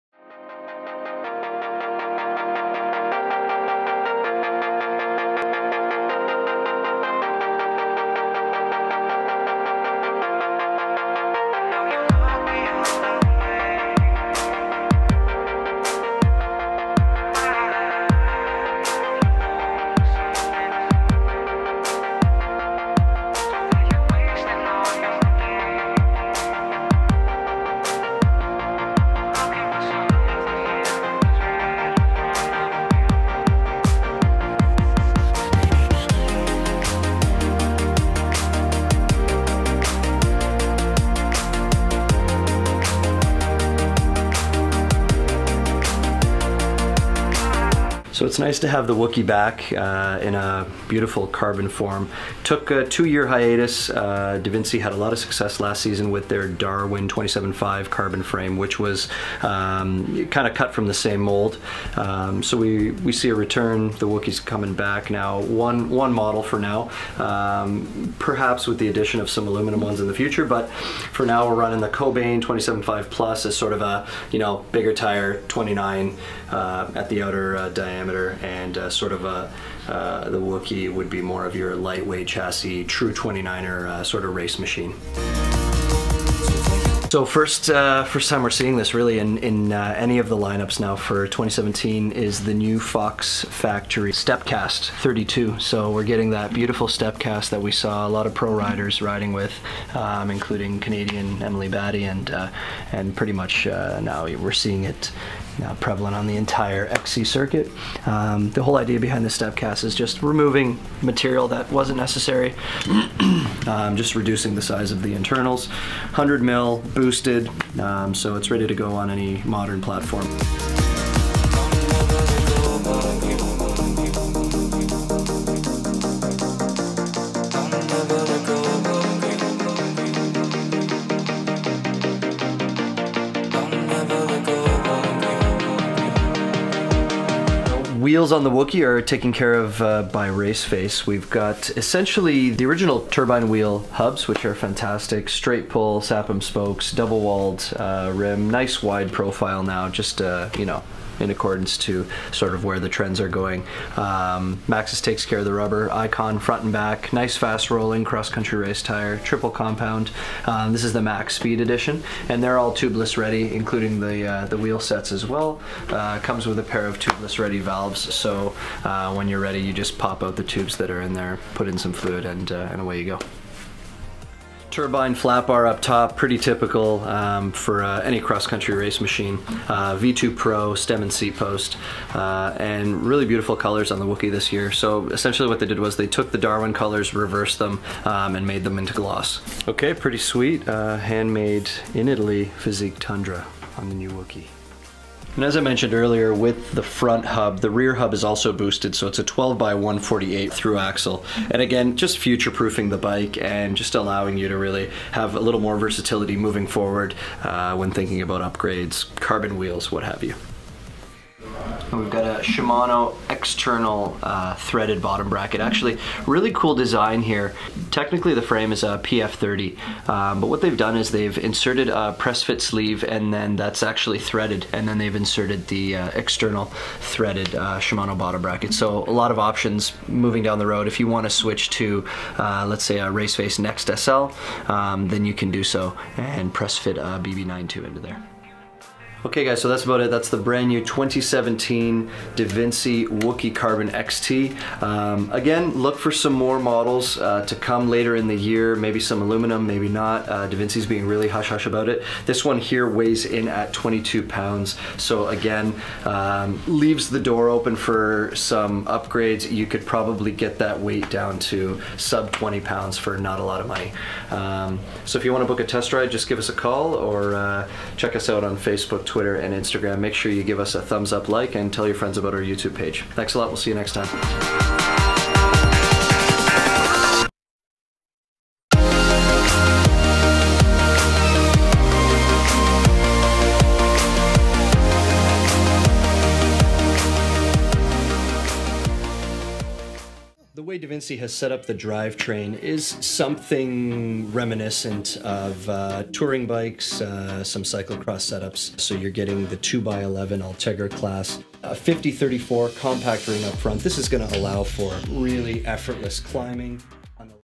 UNBOXING 004 // 2017 Devinci Wooky Carbon XT // XC Race 29er
Crushing its singletrack prey with a beastly 29er footprint, Devinci’s lightning-fast Carbon Wooky is a race champ powered by peerless handling and a superior off-road skill set. Ambitious attributes stem from Devinci’s advanced XC platform, integrating aggressive geometry with proven DMC-G carbon fiber technology. The result is a bike that levitates on the ascent, delivers hair-trigger pedal power, and tackles everything from exposed rock to roots and ruts with raw enthusiasm. Introducing the 2017 Carbon Wooky—an XC race machine making giant strides toward a podium near you.